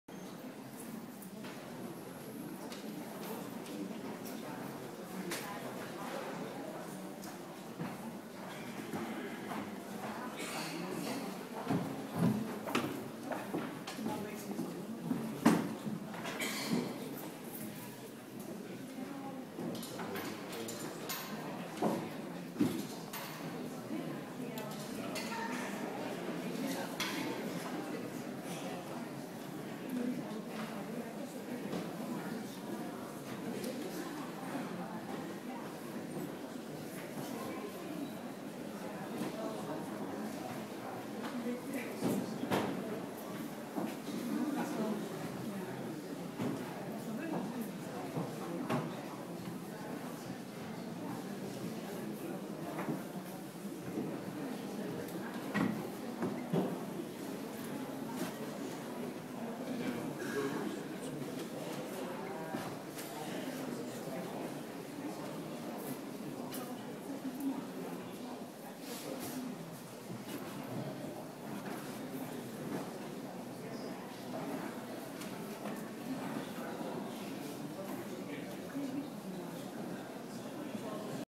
Part 2 Christmas Eve Midnight Mass in Zaltbommel
St. Martinus Catholic Church, Zaltbommel NL

Lighting candles by Mary in darkened church